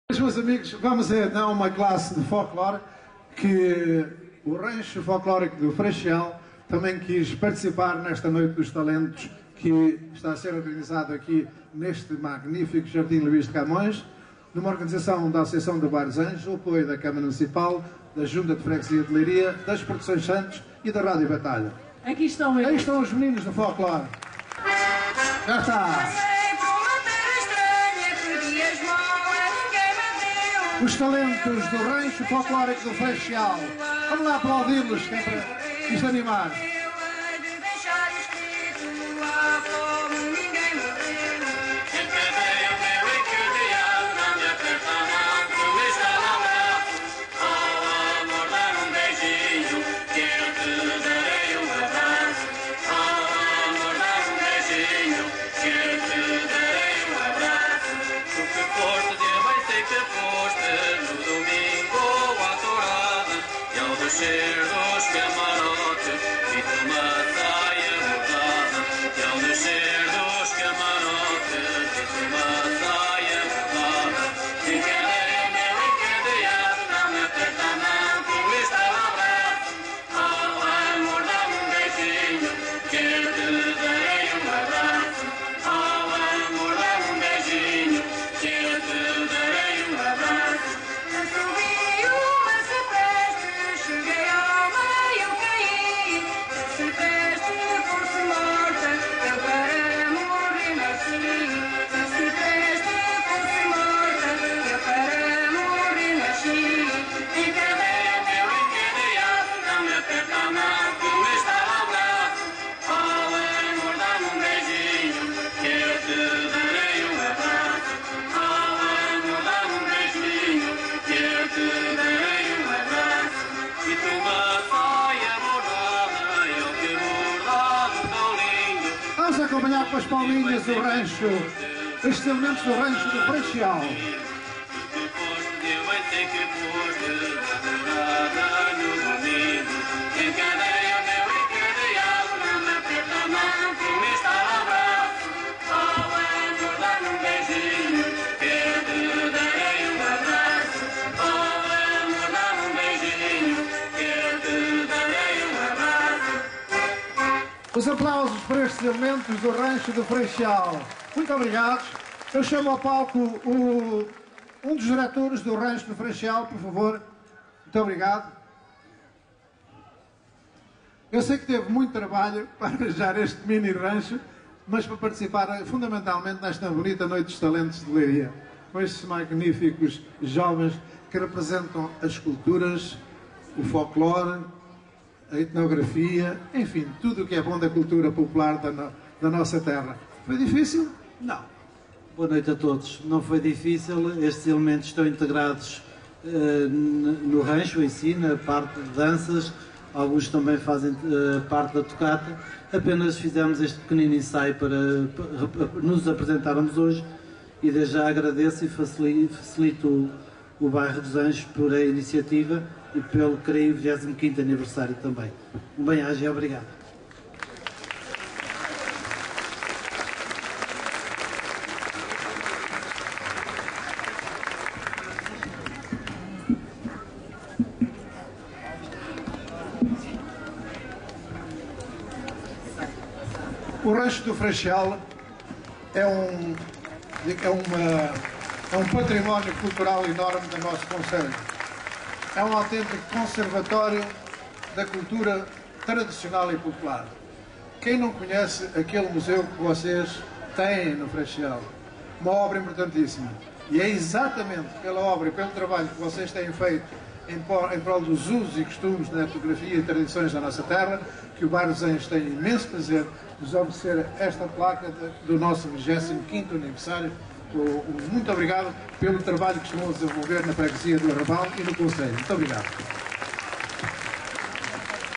PEQUENOS TALENTOS RANCHO DO FREIXIAL 7
1ªGALA DOS PEQUENOS TALENTOS DE LEIRIA
NUMA ORGANIZAÇÃO DO BAIRRO DOS ANJOS E COM O APOIO DA CÂMARA MUNICIPAL DE LEIRIA, JUNTA DE FREGUESIA DE LEIRIA, PRODUÇÕES SANTOS E RÁDIO BATALHA TEVE LUGAR NO JARDIM LUIS DE CAMÕES A 1ªEDIÇÃO DA GALA DOS PEQUENOS MAS GRANDES TALENTOS.
APRESENTAÇÃO: MANUELA OLIVEIRA E JOAQUIM SANTOS
ORGANIZAÇÃO: COLECTIVIDADE DO BAIRRO DOS ANJOS COMEMORANDO OS SEUS 25 ANOS.